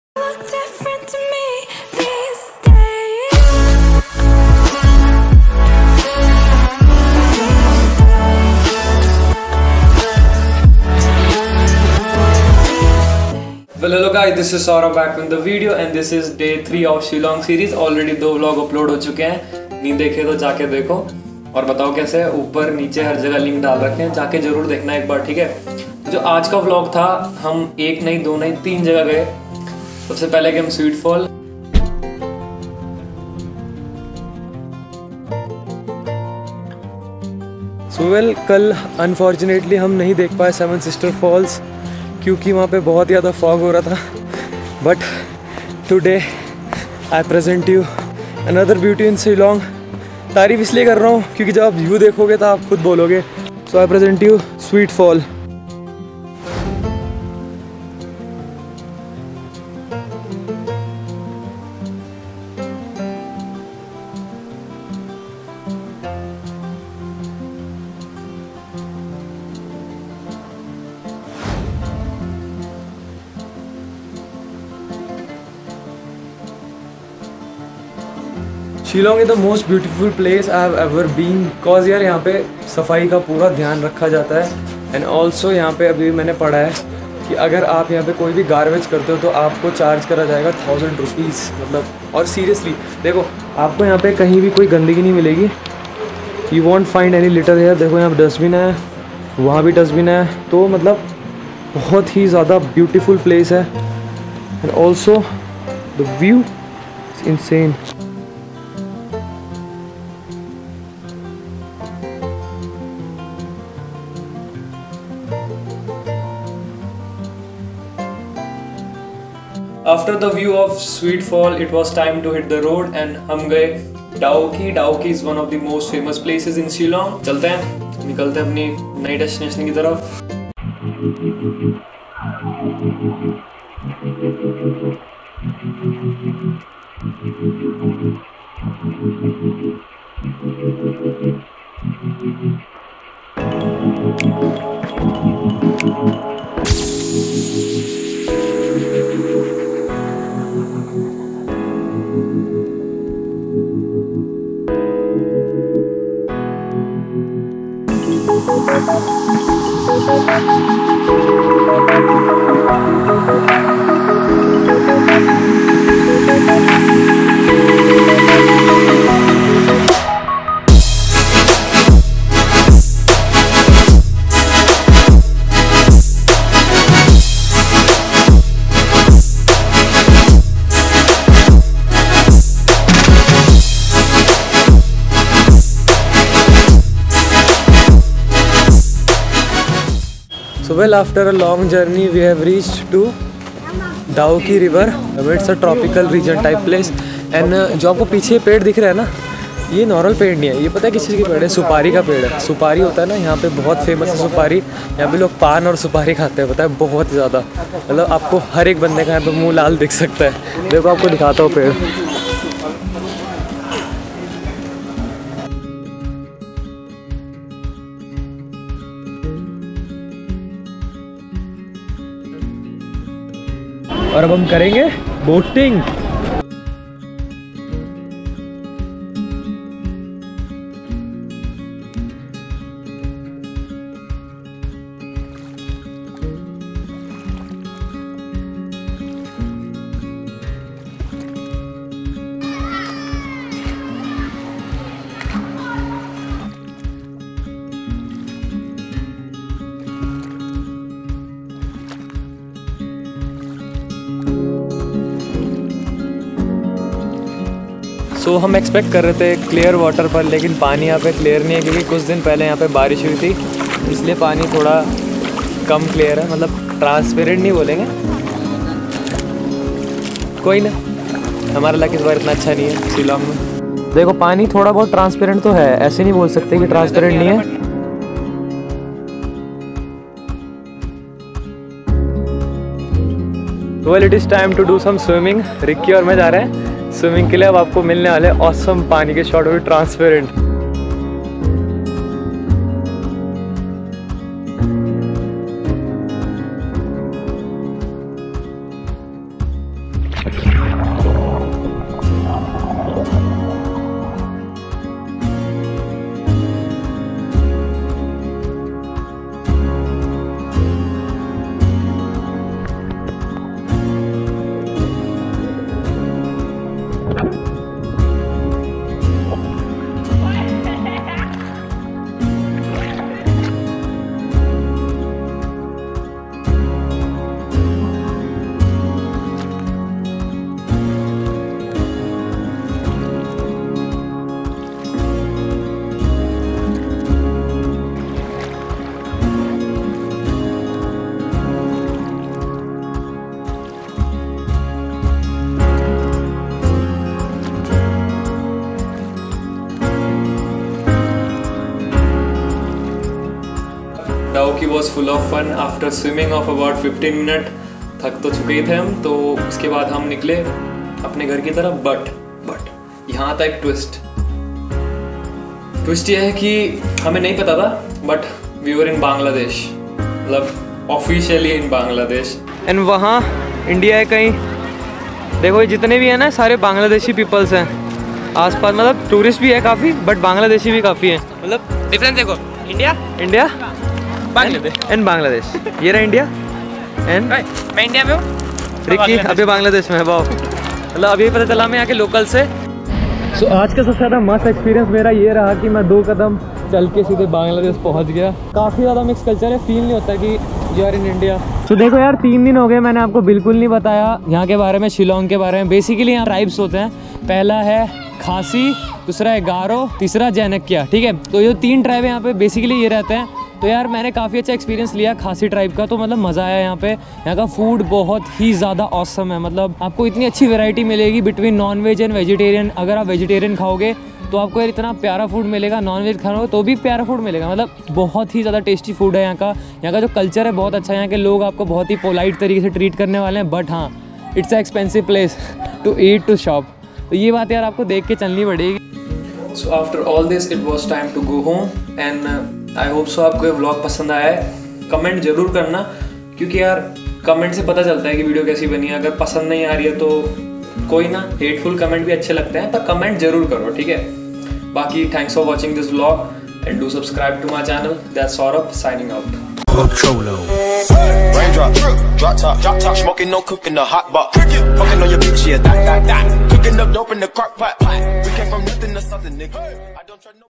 Exploring The Transparent River | Dawki
This summarise our day 3 in shillong
We visited Sweet fall very close to the main city but the view is breath taking

Then we decided to go
Dawaki known as transparent river

The best part of the day was knowing the fact that we entered Bangladesh while boating in dawaki river